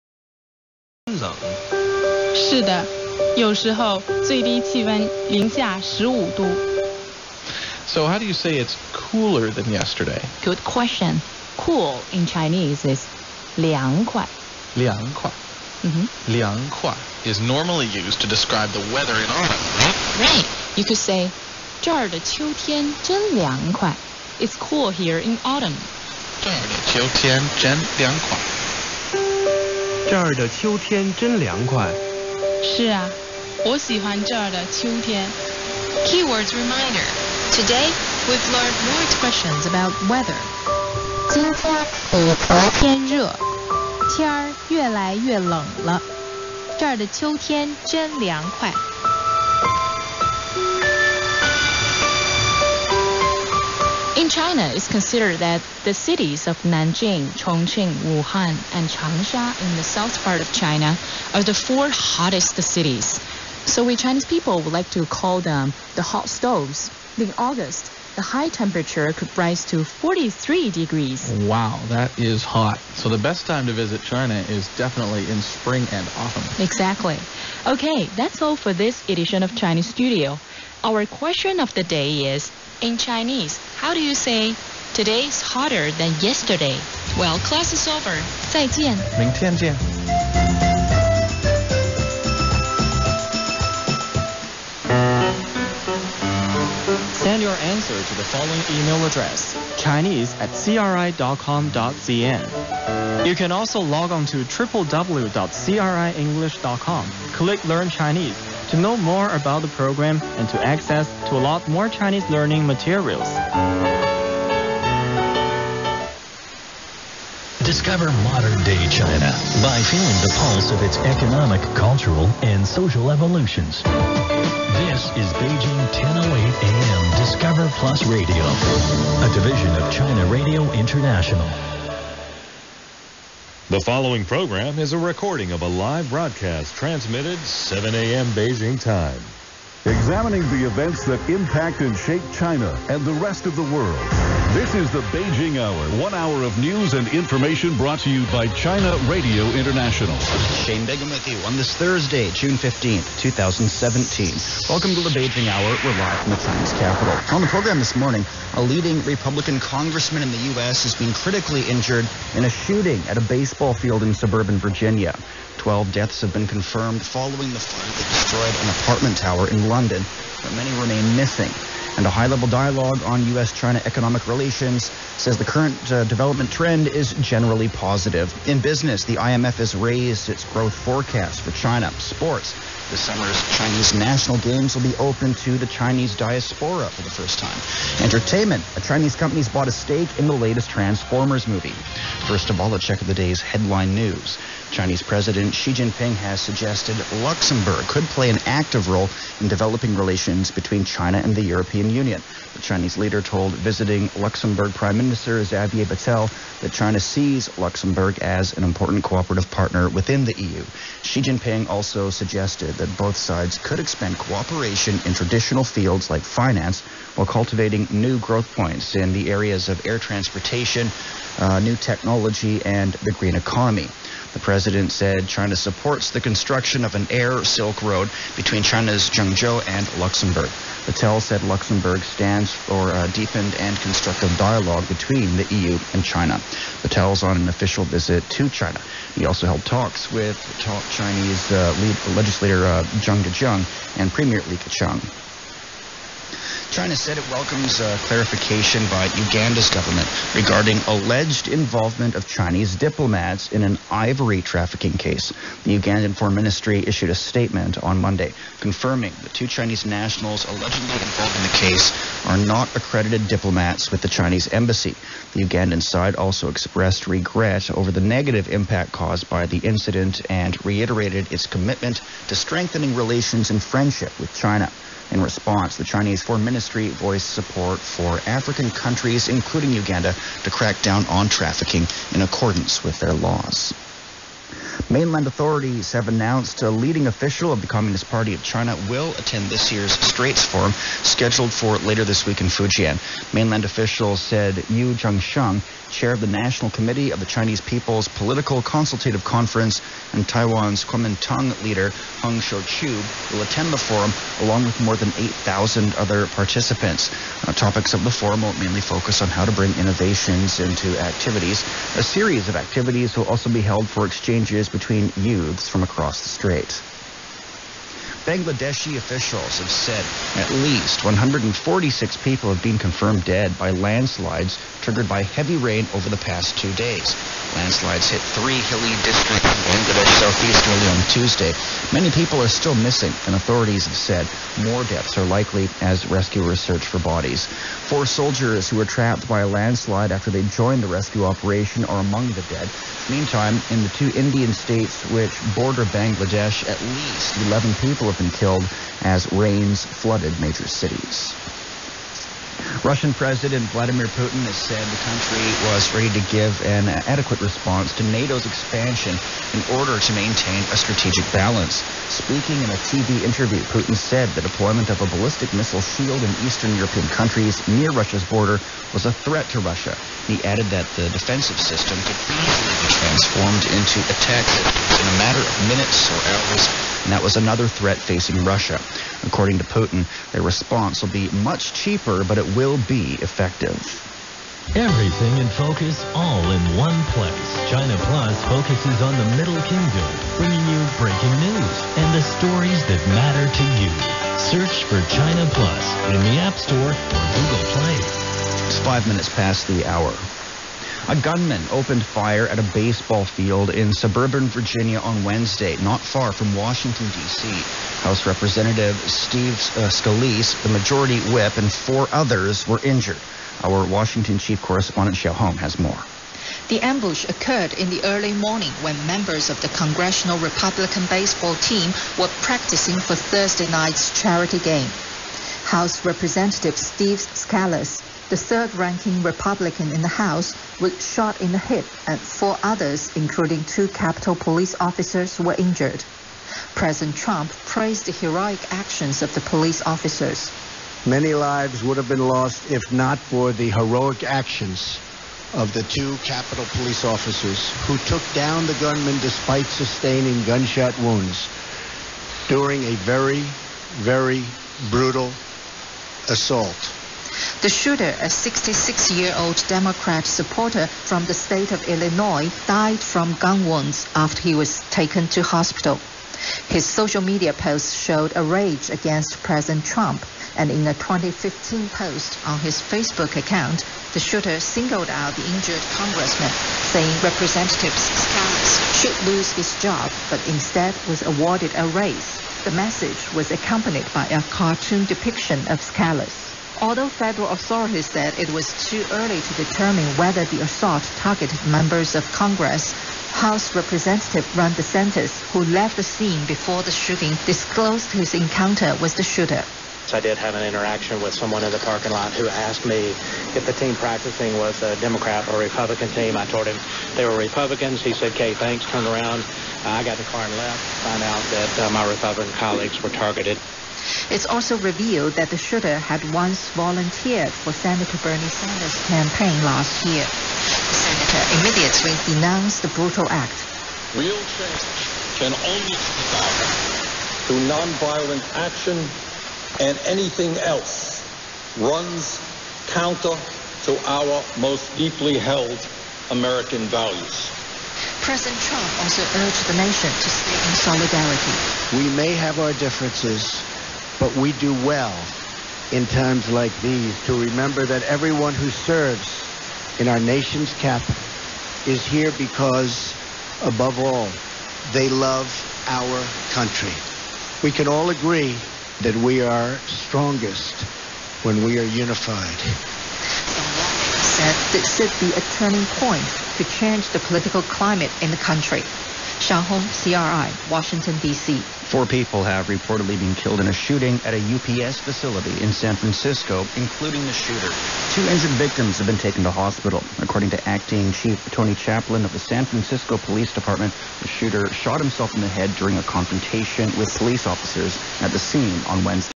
Ham Radio Fun: China and the EU, Ivory Trafficking, Donald Trump Audio, Shooting in Virginia
China Radio International with China and the EU, Ivory Trafficking, Vladimir Putin, Donald Trump, Shooting in Alexandria and more.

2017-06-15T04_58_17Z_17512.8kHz

Shortwave Capture by Cheaptoyman